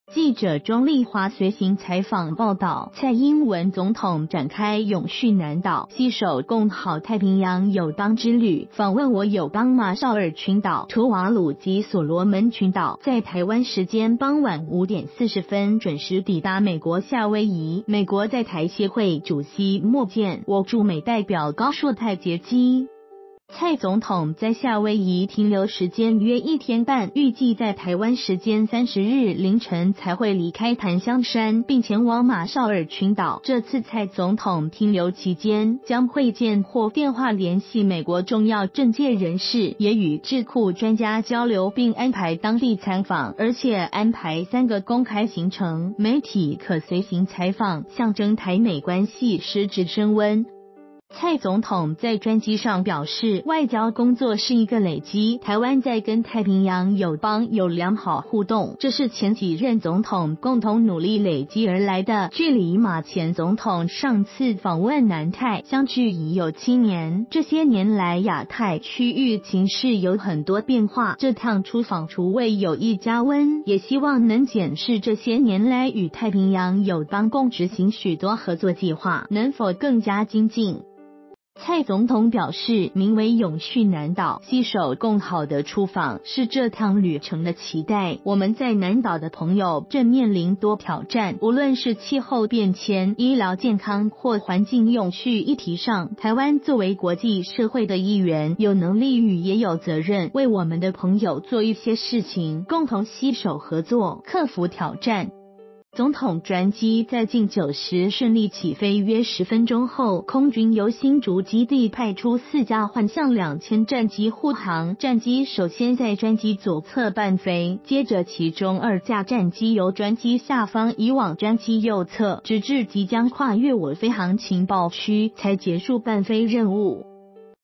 蔡總統出訪抵夏威夷 停留1天半後前往馬紹爾
〔記者鍾麗華／隨行採訪報導〕蔡英文總統展開「永續南島，攜手共好」太平洋友邦之旅，訪問我友邦馬紹爾群島、吐瓦魯及索羅門群島，在台灣時間傍晚5點40分準時抵達美國夏威夷，美國在台協會主席莫健、我駐美代表高碩泰接機。